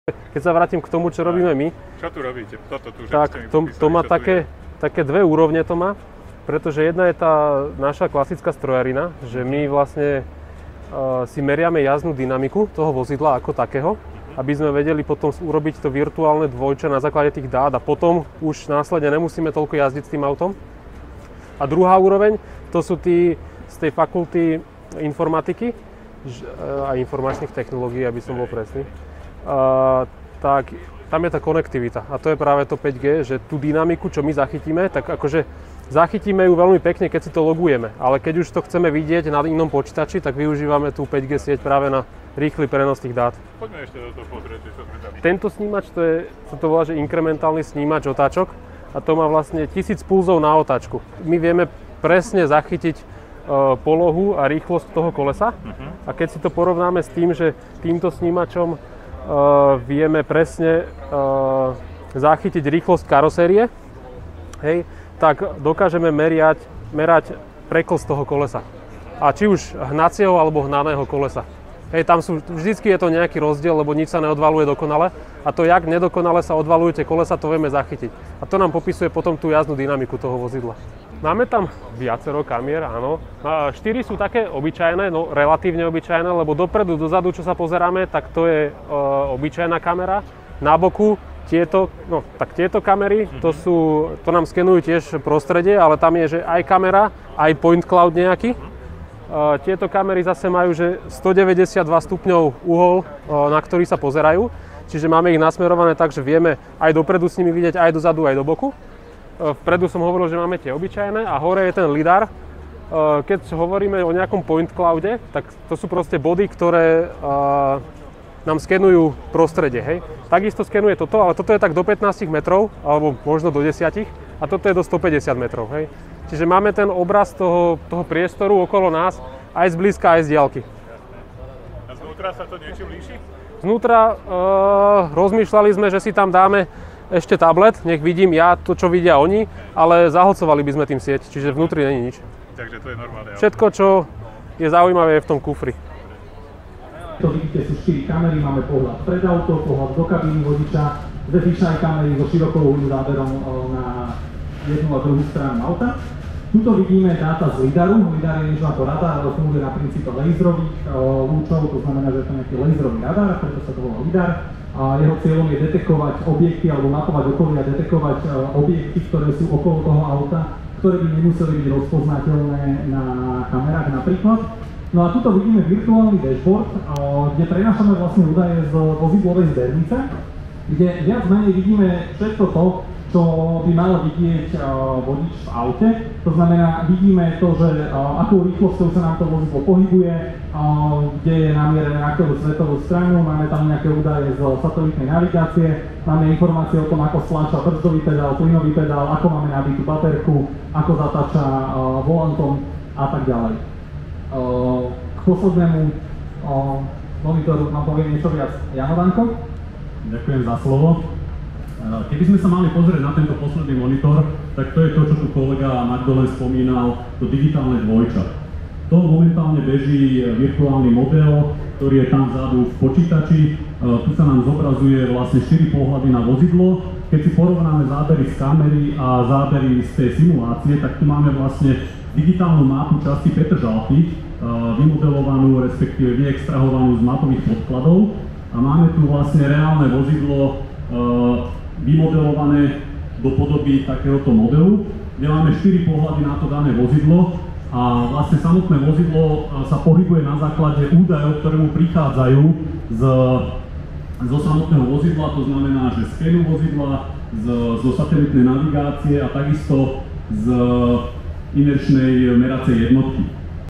Auto s prenosom dát cez 5G sieť už jazdí v Bratislave
Auto s nainštalovanými snímačmi a kamerami sa pohybuje po Bratislave. Všetky údaje sa prenášajú cez 5G sieť a partnerom je Orange. Pozrite si detaily.